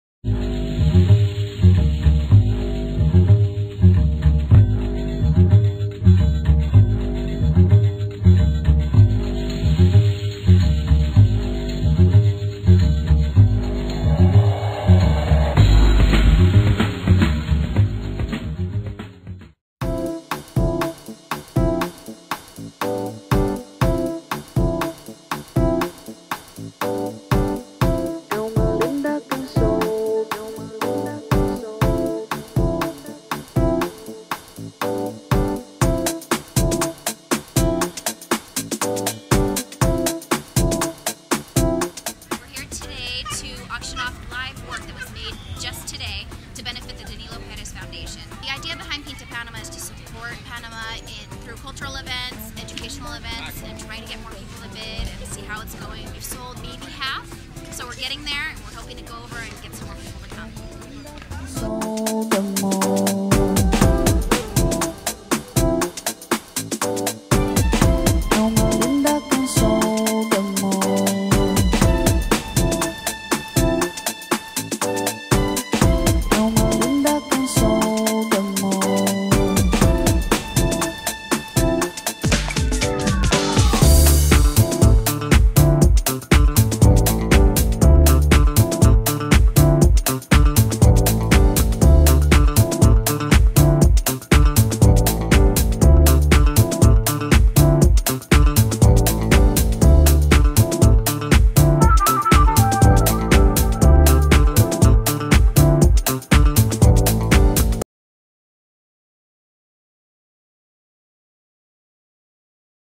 Panama Jazz Festival Casco Viejo, Panama City: Arte en Vivo by Pinta Panama
recorded one of Pinta Panama's first fundraising events.  "Arte en Vivo" was an art auction for the Danilo Perez Foundation held in Casco Viejo, Panama City.  The event took place during Jazz Festival, one of Casco Viejo's largest cultural events.  The event was an great success.  To learn more information regarding upcoming fundraising events, please contact